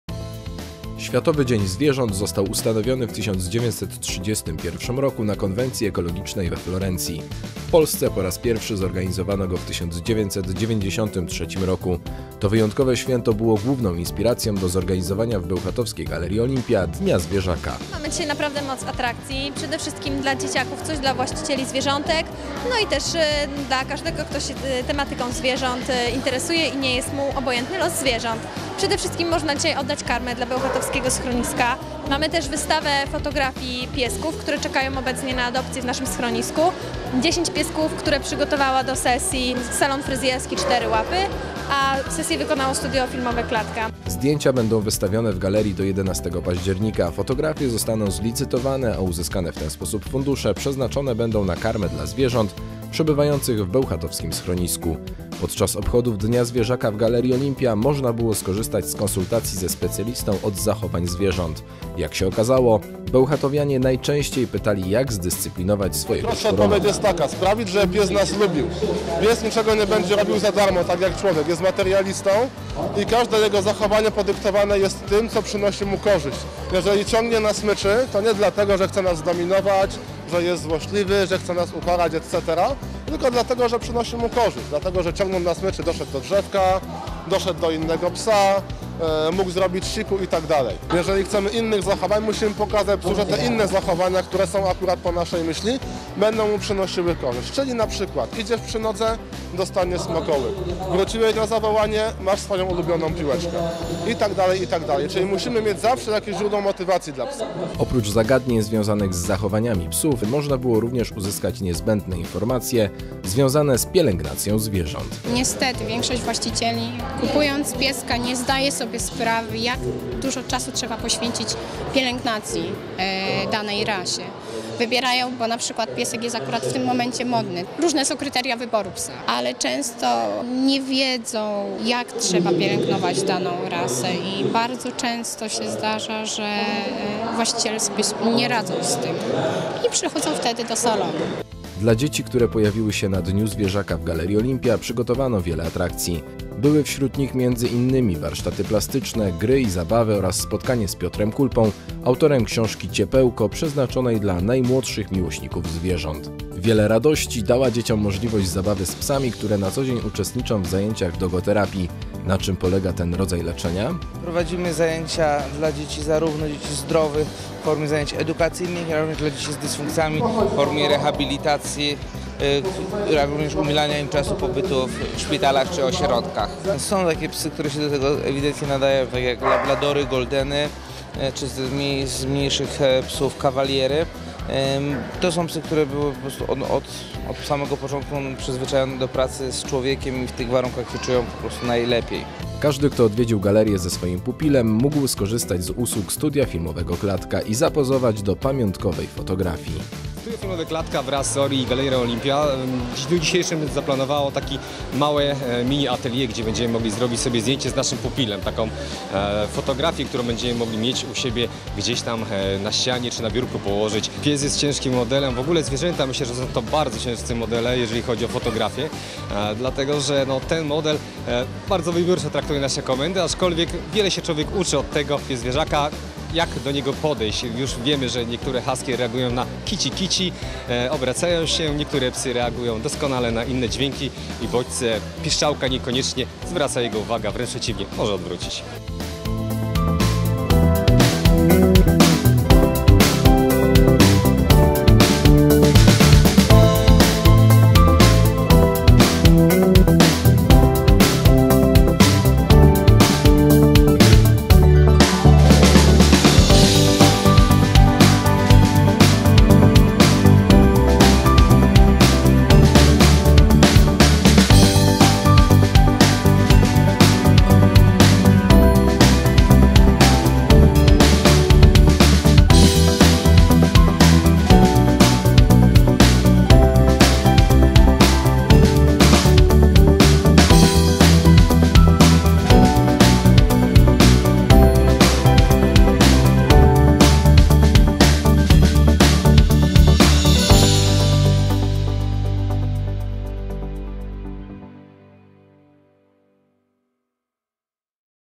Dzień Zwierzaka w Galerii Olimpia w Bełchatowie
6 października w Galerii Olimpia obchodziliśmy Światowy Dzień Zwierząt.